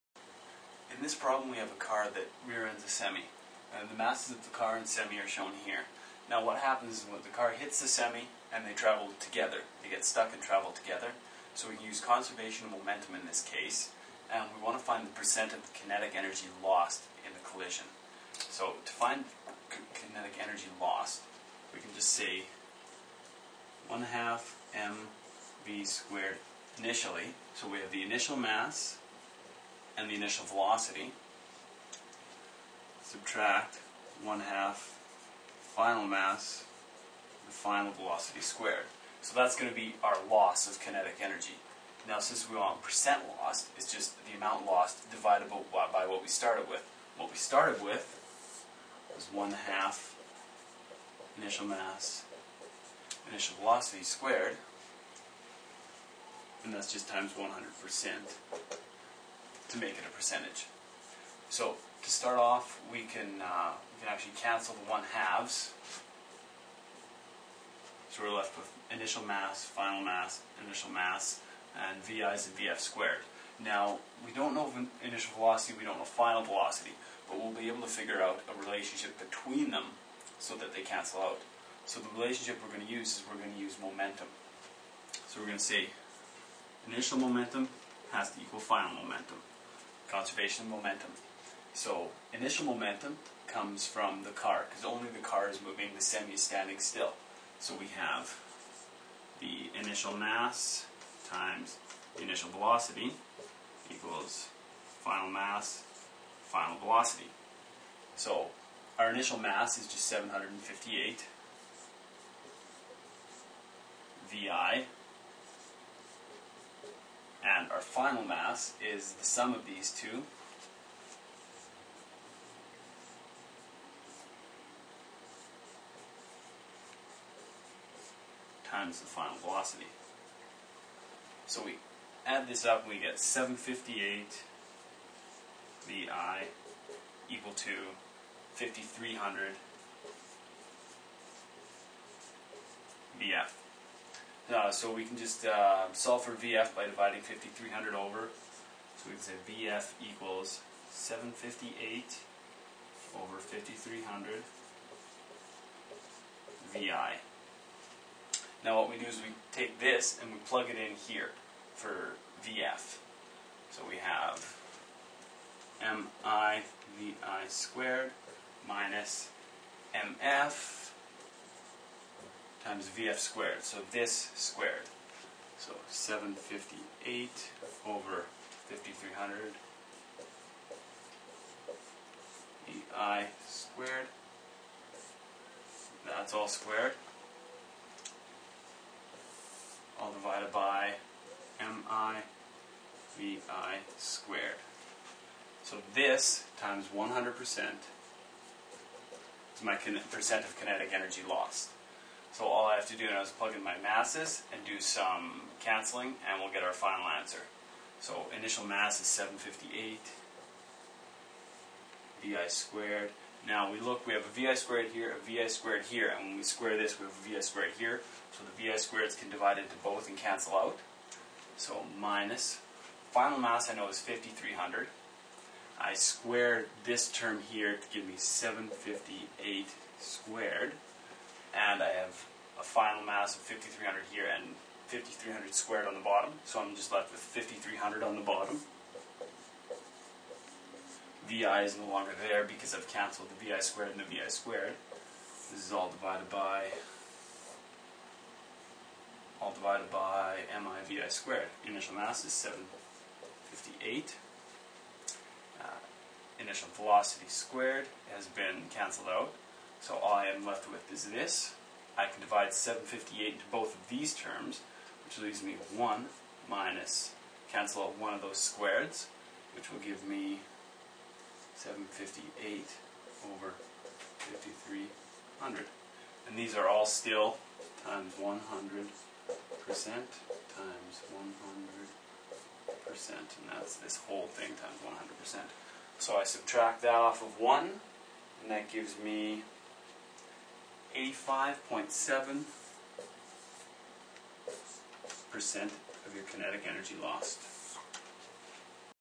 Impulse-Momentum - Rear end collision
A 758 kg car rear ends a 4542 kg semi that is sitting in neutral at a red light.  The vehicles get stuck and travel forward together afterwards.  What is the percentage of kinetic energy lost in this collision?